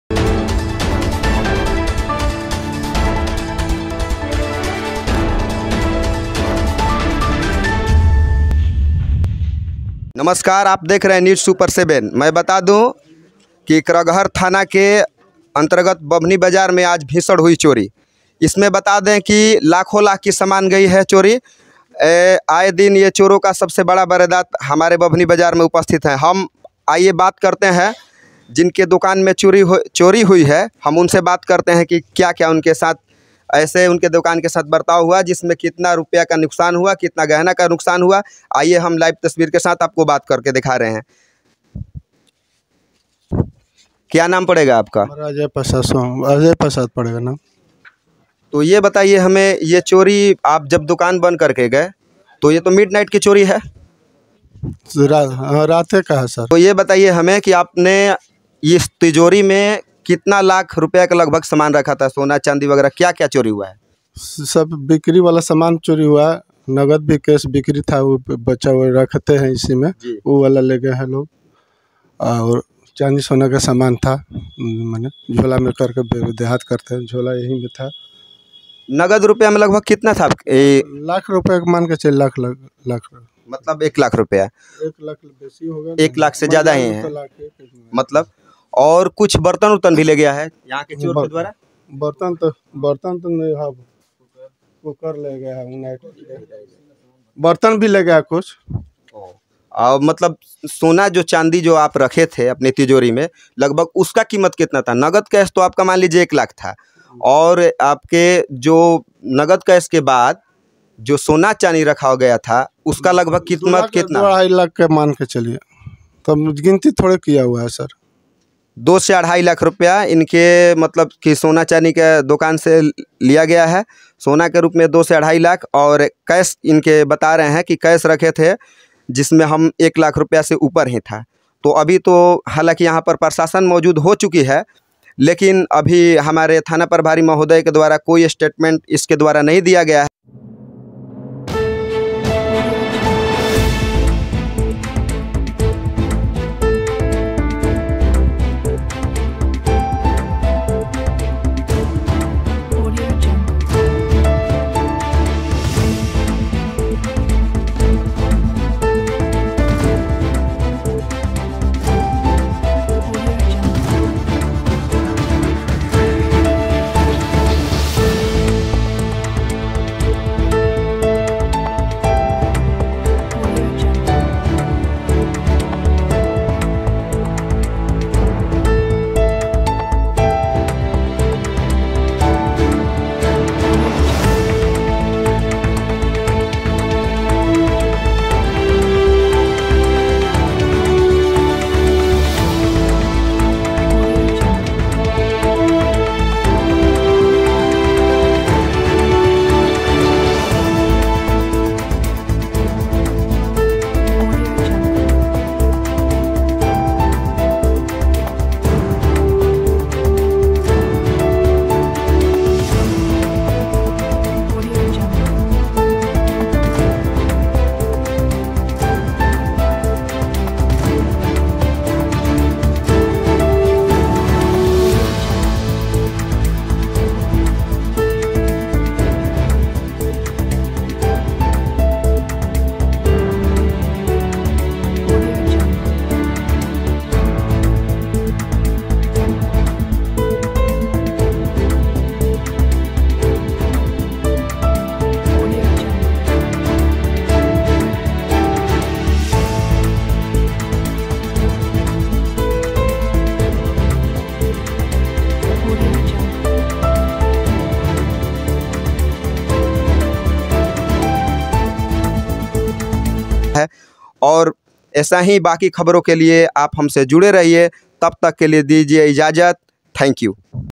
रातों रात लाखों का सामान उड़ा ले गये चोर, रोहतास में बढ़ता जा रहा चोरी और मर्डर जैसे अपराध
Vedio credit:
Reporter : रंजित कुमार
Edited by:  Kumar Sudhansu

आप हमे follow करें ताकि हर पल हर खबर आप तक मिलता रहे :-